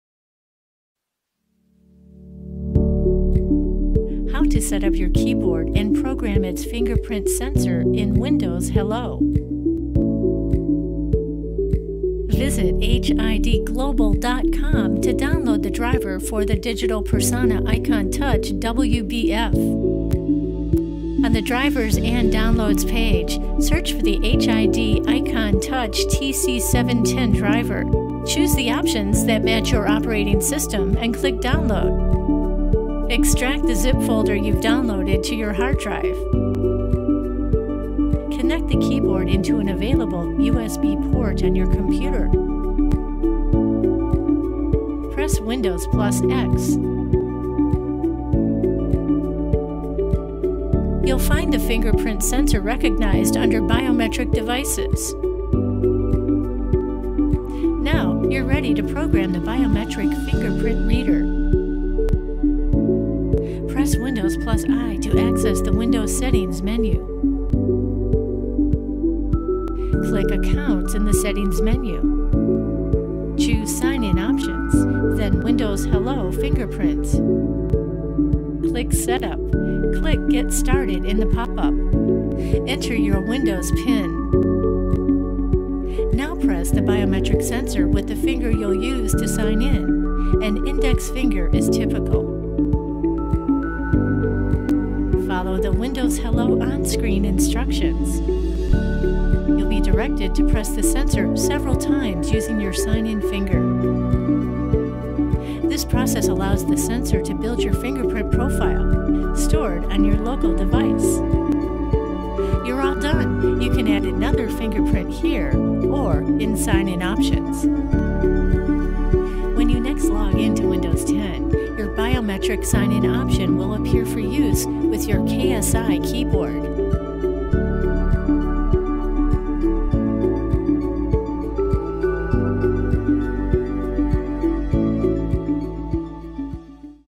Setup for HID EikonTouch Fingerprint Sensor, Method II
How to set up the HID EikonTouch Fingerprint Sensor on your KSI keyboard.